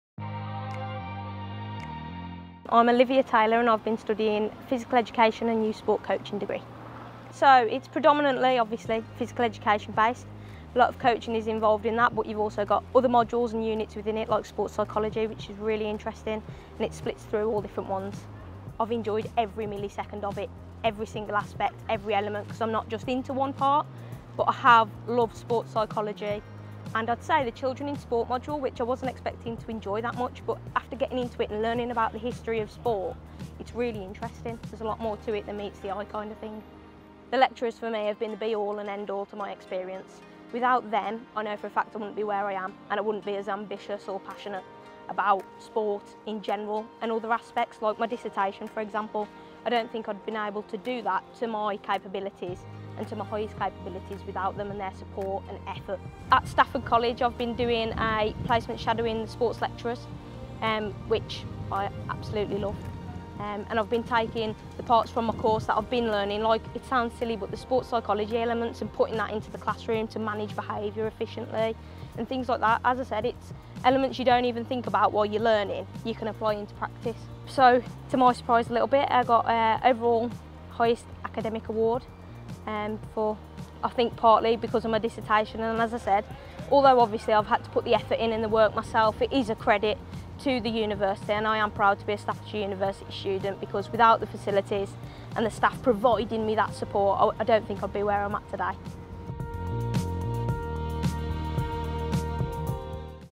BSc Physical Education and Sports Coaching - Olivia Taylor
Olivia was awarded an Academic Achievement Award after graduating with a First in Physical Education and Youth Sport Coaching (now Physical Education and Sports Coaching).

Hear about what her favourite part of the course was, and the impact the lecturers had.